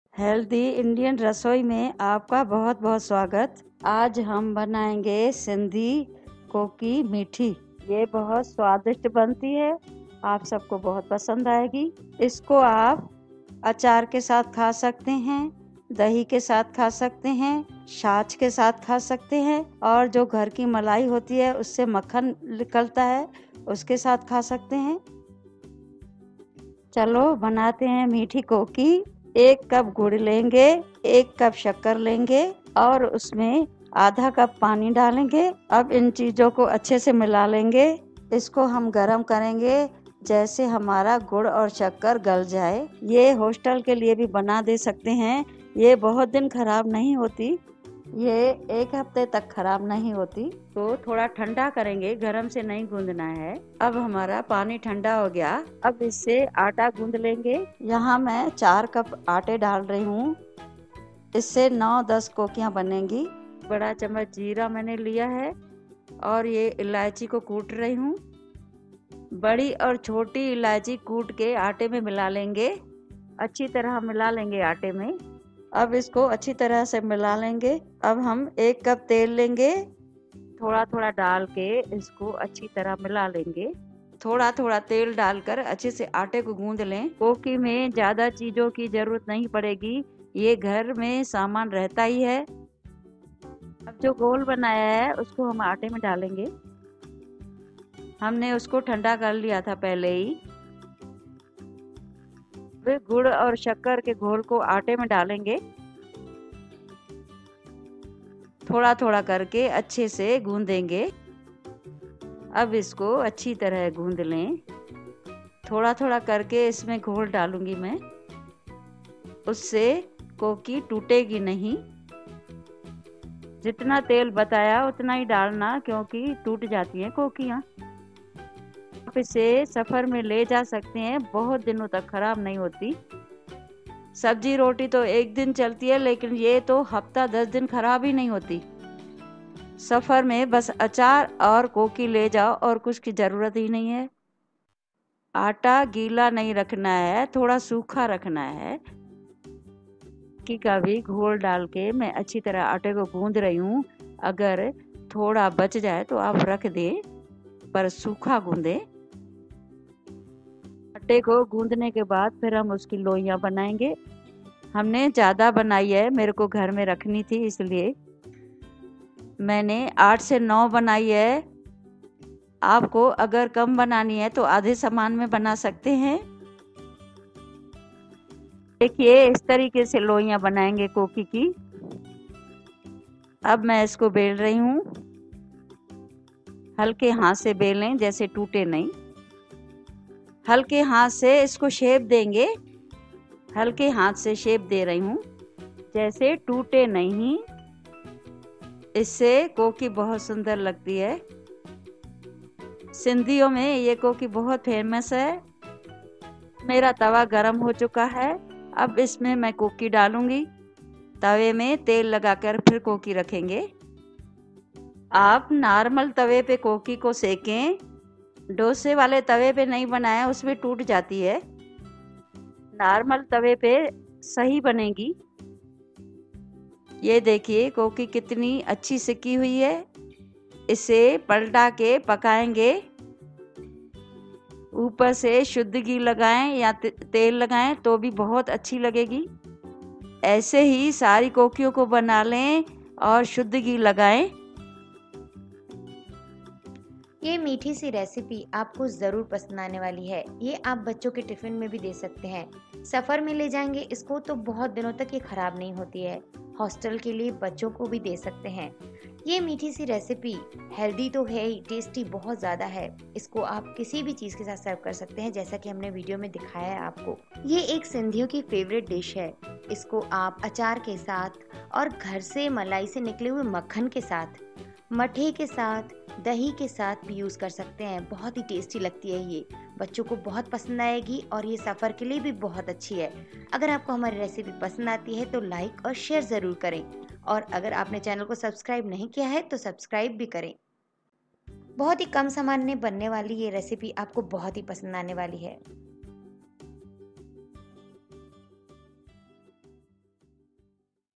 सिंधी ❤️Koki मिठो लोलो आज बनेगी हमारी रसोई में Sindhi Special असली कोकी गुरनानी साथ बनायें थदड़ी पे
Other Breakfast Recipes:-